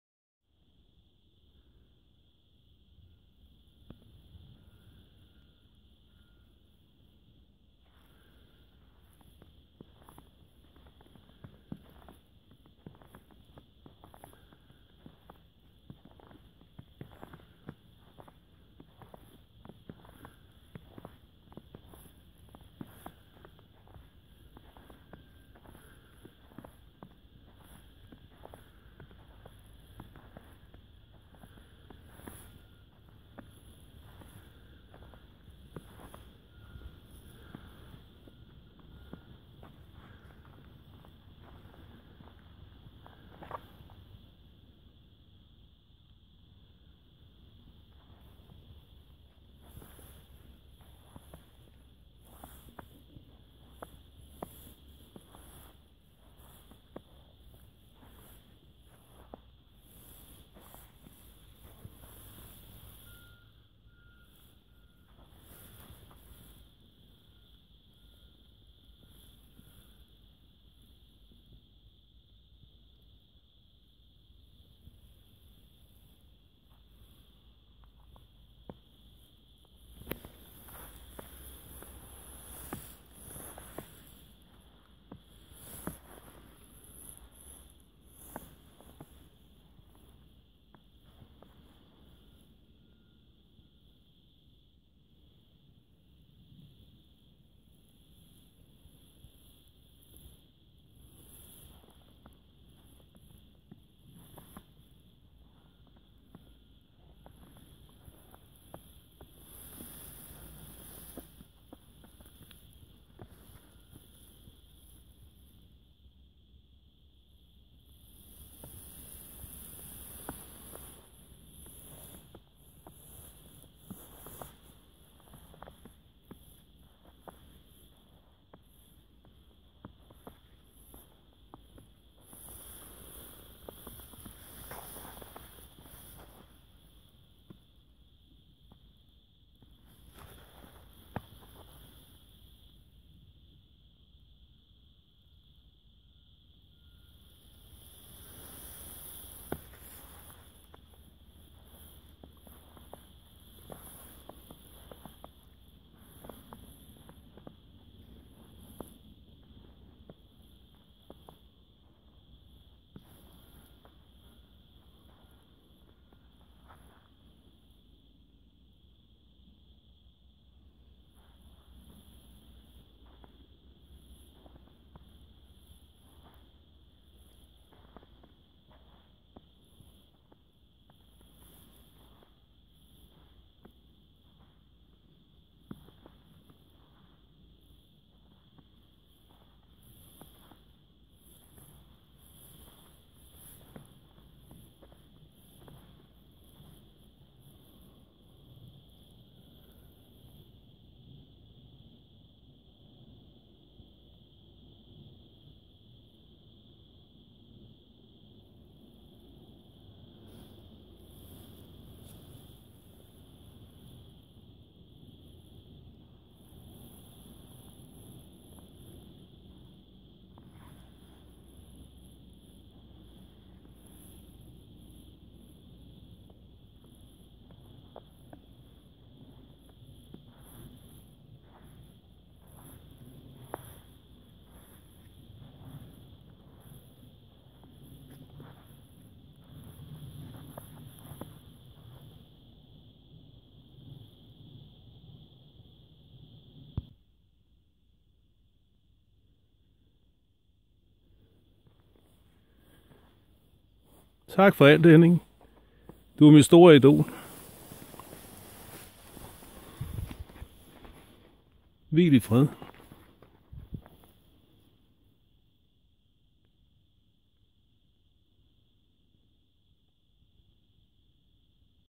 Henning Jensen The Football Legends Burial - RIP - Recorded 11 Dec. 2017 Denmark
See the video in 1080p HD -
Henning Jensen
Born 17. august 1949 -- Died 4. december 2017
68 years old.
Henning Jensen was my big Football idol when i was a kid and playd Football. I was so Lucky to see him playing for
Borussia Mönchengladbach, against KB ( Today FCK )
Henning Jensen and Pelé was my favorites.
Henning Jensen was burried the 9 december 2017 in Hammer Church. Hammer Bakker, North Jutland, Denmark. Deep inside a very beautyful forest, not far away from where he was born "Nørresundby".